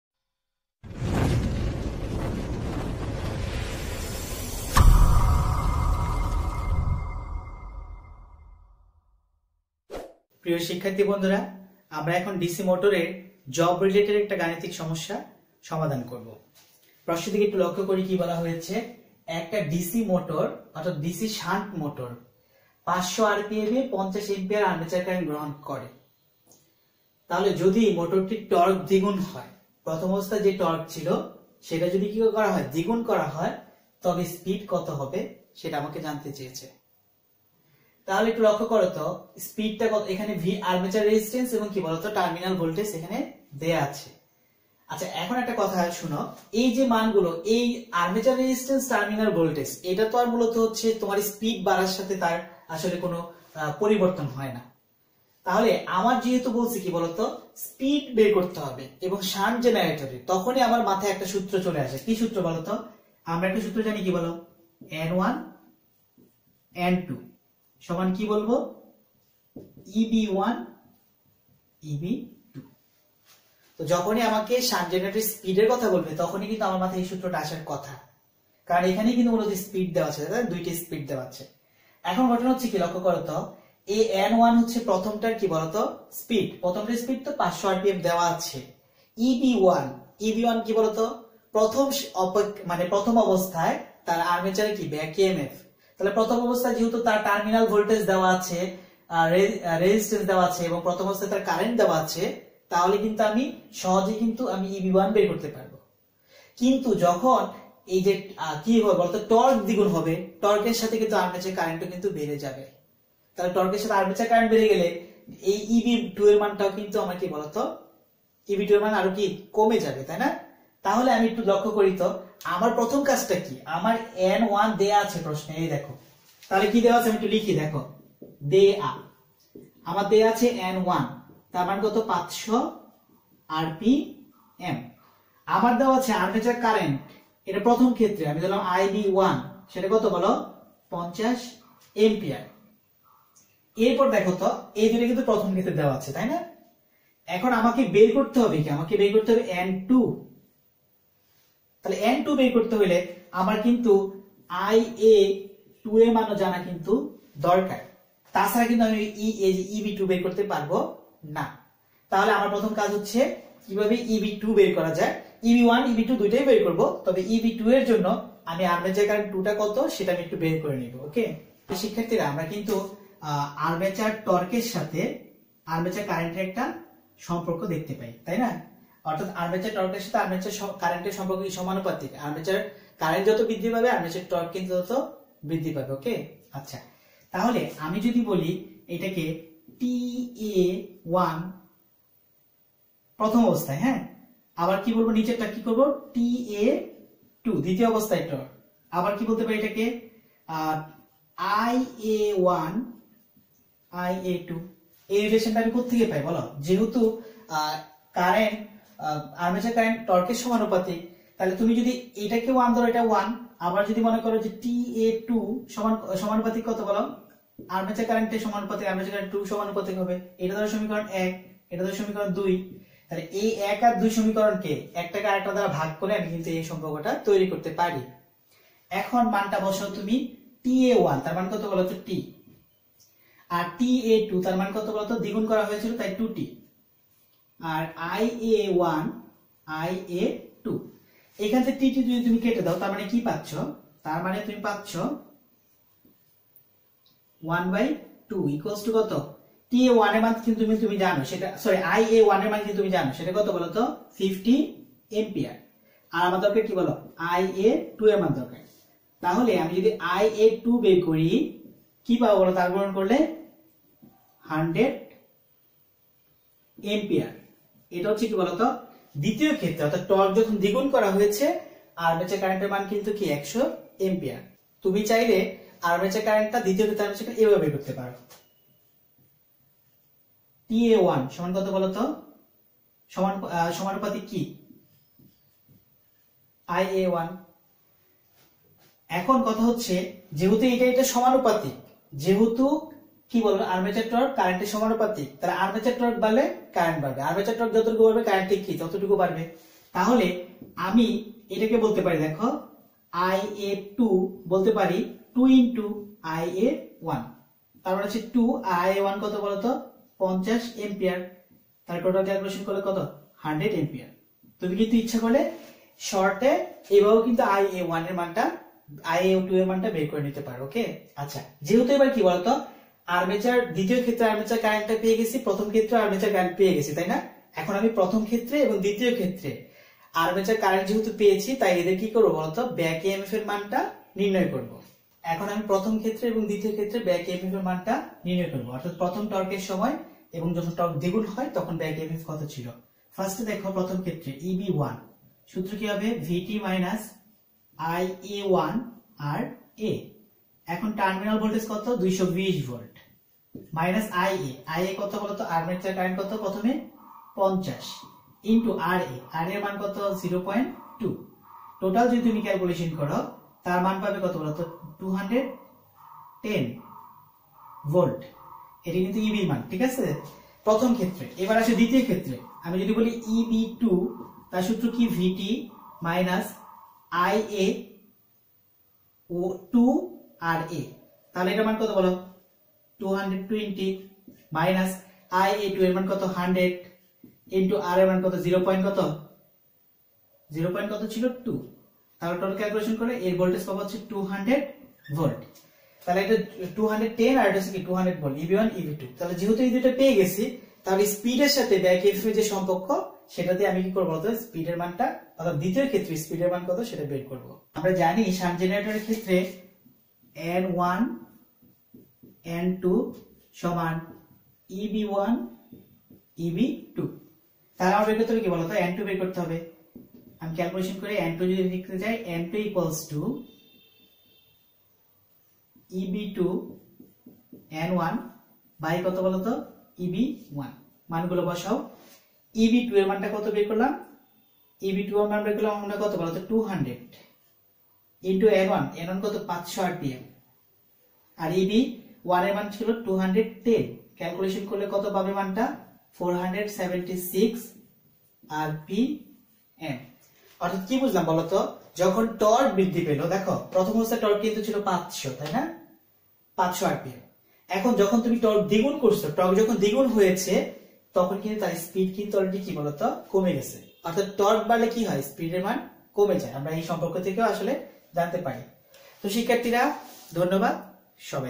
DC Machines|| Job Part-06|| আজিজুল হক || EEE Guru BD
প্রশ্নঃ একটি ডিসি শান্ট মোটর 500 rpm এ 50A কারেন্ট গ্রহণ করে। যদি মোটরের টর্ক দ্বিগুণ করা হয় তবে স্পিড কত হবে?[Ra= 0.2 ohm; Vt=220V]

Q: A DC shunt motor receives 50A current at 500 rpm. If the torque of the motor is doubled, what is the speed? [Ra = 0.2 ohm; Vt = 220V]